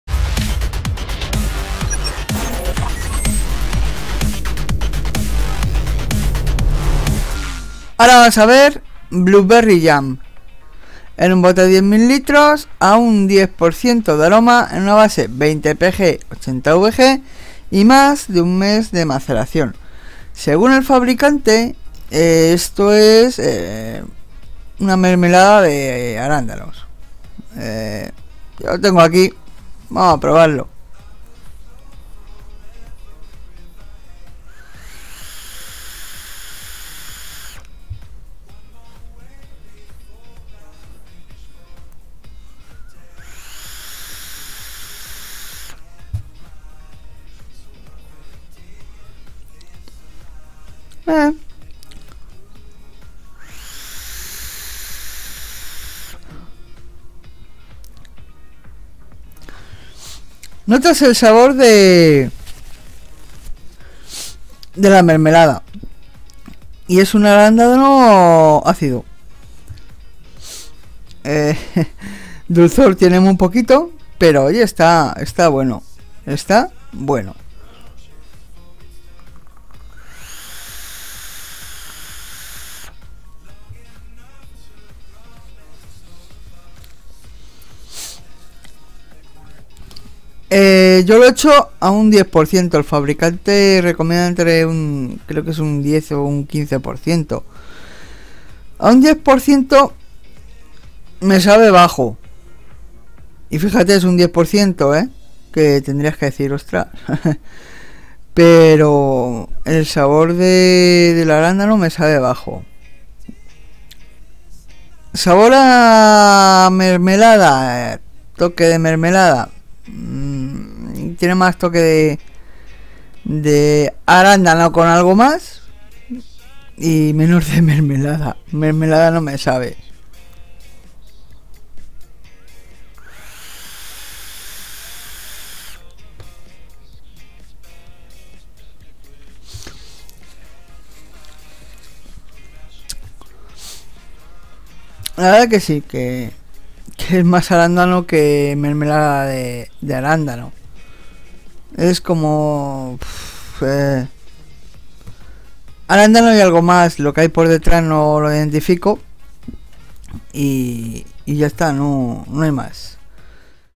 Revisión de aromas Capella: Blueberry Jam
Hoy revisamos esta mermelada de arandanos de Capella.

Un aroma que esperaba mas de él, ya que solo sabe a arandanos con un toque ácido y algo mas por detrás que no identifico.

El sabor a mermelada para mí, es inapreciable o se les ha olvidado ponerlo.

El fabiricante recomienda:

Tiempo de maceración: 20-30 Días

La revisión esta recializadas:

Recipiente: 10 ml
Base: 20 PG/80 VG
Porcentaje utilizado: 10%
Tiempo de maceración: 30 Dias